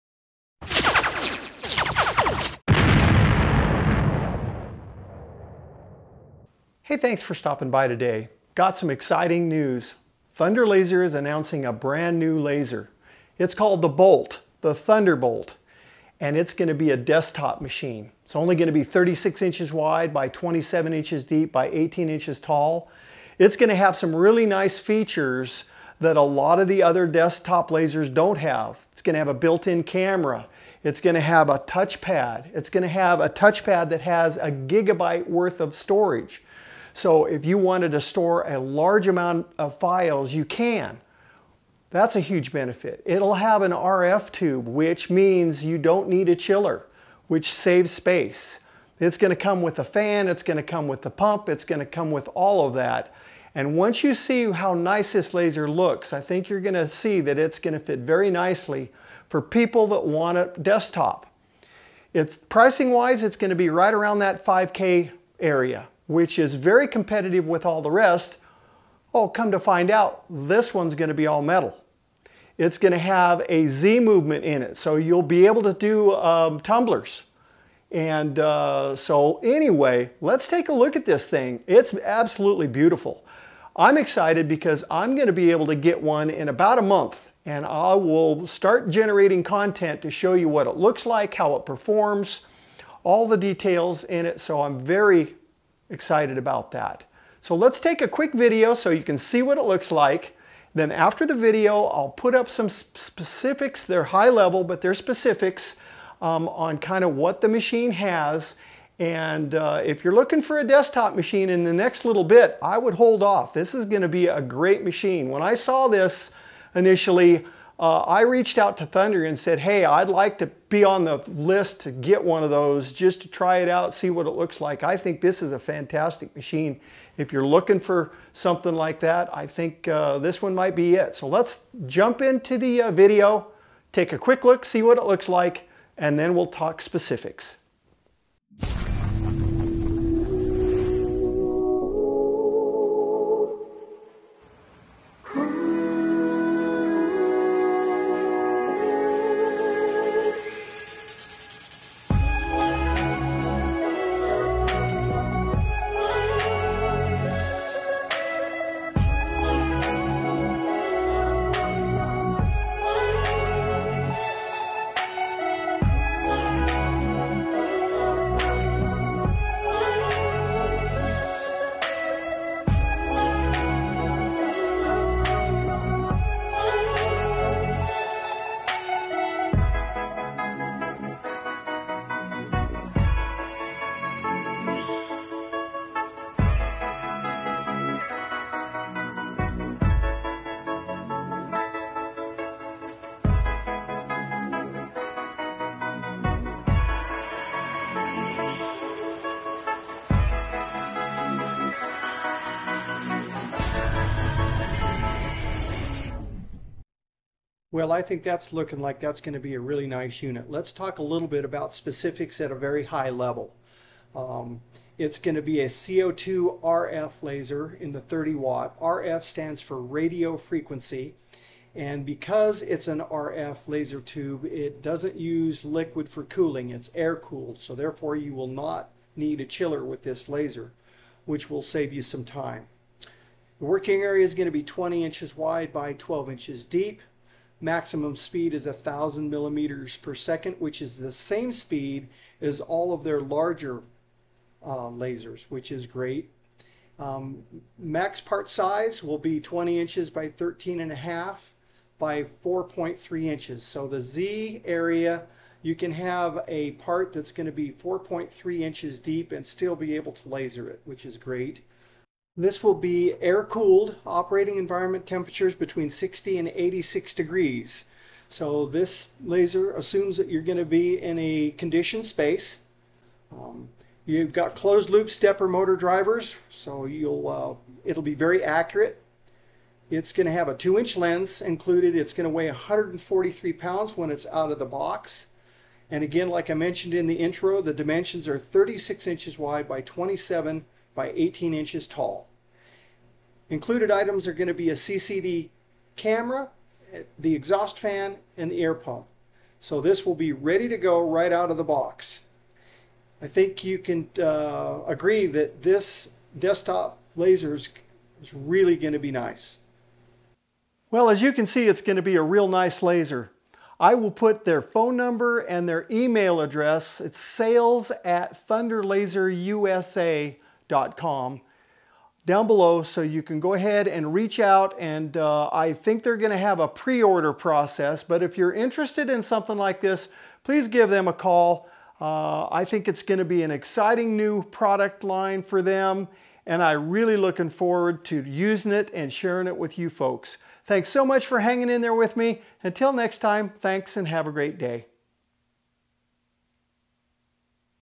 Thunder Lasers New Bolt Desktop RF Laser. Updated Specs in Description!
In this video I discuss the all new Thunder Bolt Desktop laser. This new desktop laser has allot of great features not found on allot of other models in this size and price range. This new class of laser for Thunder will definitely provide a top quality laser for people that need a smaller laser footprint with big capabilities.

A big thanks to Chris Locascio for the great first look video included in this video!

Updated specs for the Thunder Bolt Laser
Working Area 20” x 12” / 508 x 305mm
Table Size 21.9” x 14” / 557 x 357mm
Z Axis 4.7” 110 mm
Max Speed 1000mm
Max Work Size 20.3 x 12.3 / 513 x 313mm
Motor - Stepper Motor
Working piece Table - Honeycomb
Max Table Load - 10 kgs
Net Weight 77 kgs
Standard Head - 1.5” (38.1mm)
Lens Diameter - 20mm
Mirror Diameter 25mm
Beam Combiner Diameter 25mm
Laser Dimensions. 37”x 28.5”x 19.6”/ 941 x 724x x 498 mm
Max part size with door closed - 20.3” x 13.9” x 4.3” / 515 x 345 mm

Thunder Laser USA  web site
Thunderlaserusa.com